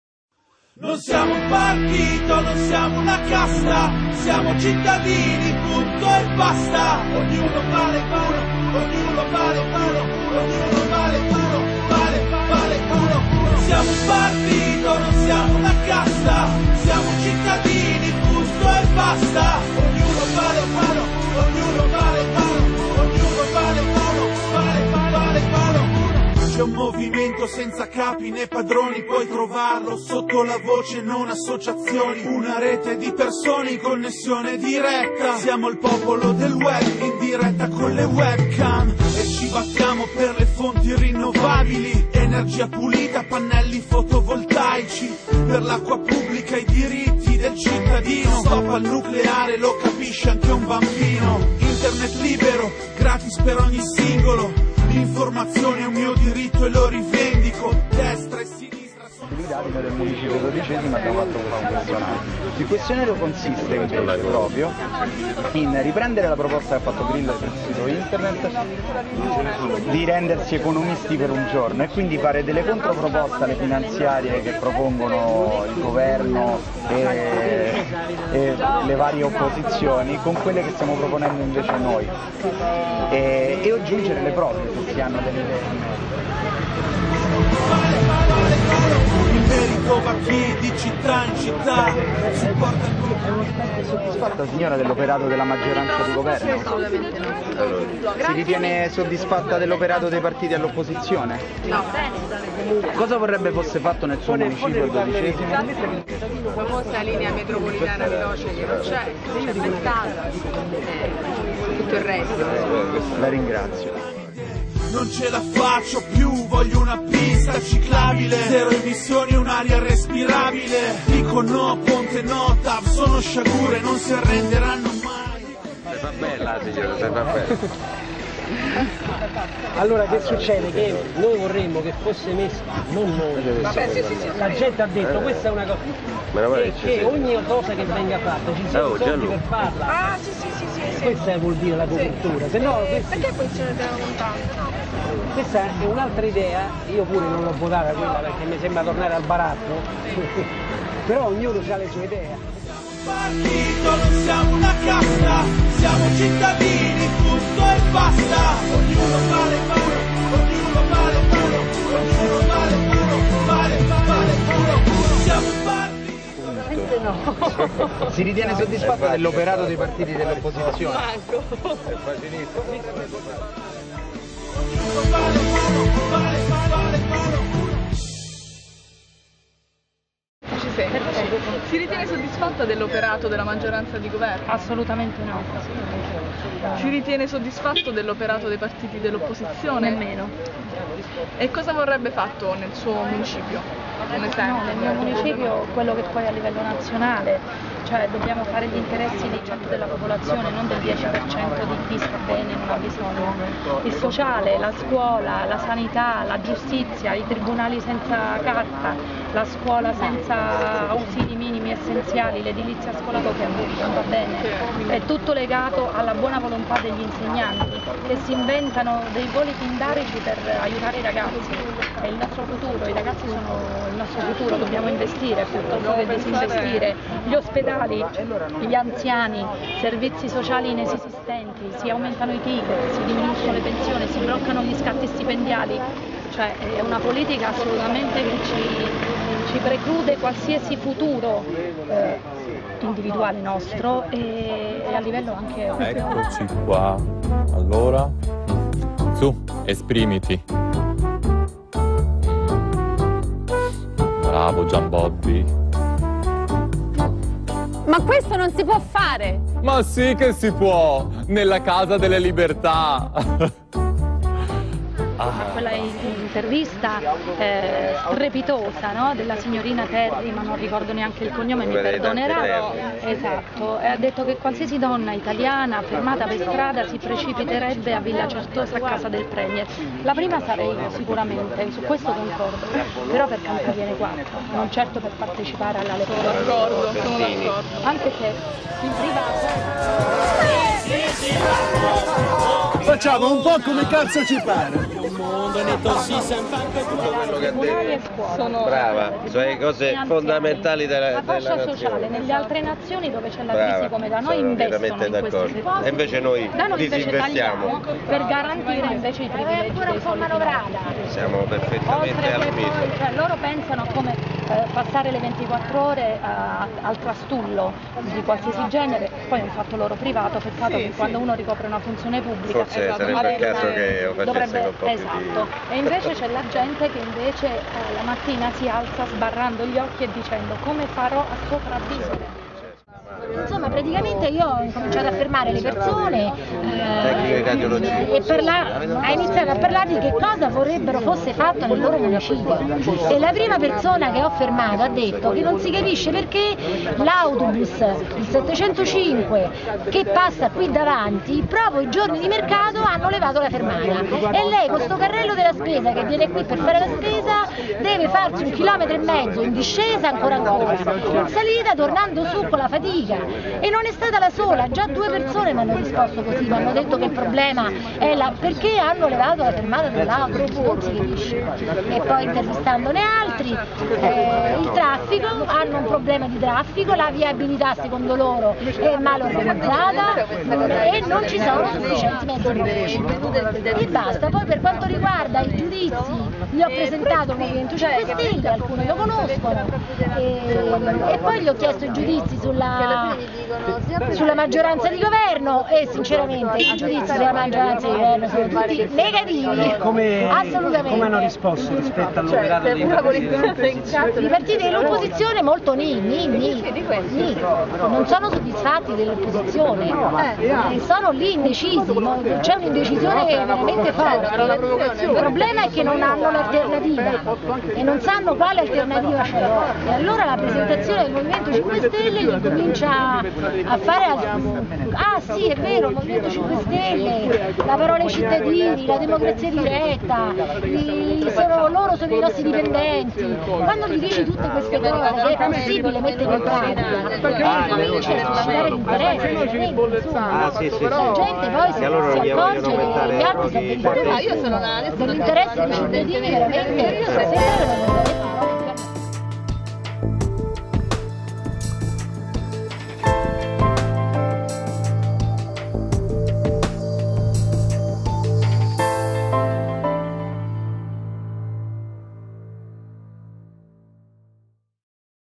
Il banchetto del M5S al mercato di Spinaceto.
Il banchetto del M5S al mercato di Spinaceto. Interviste e immagini prodotte dai sostenitori del M5S che invitano i cittadini a mobilitarsi attivandosi per una politica PULITA.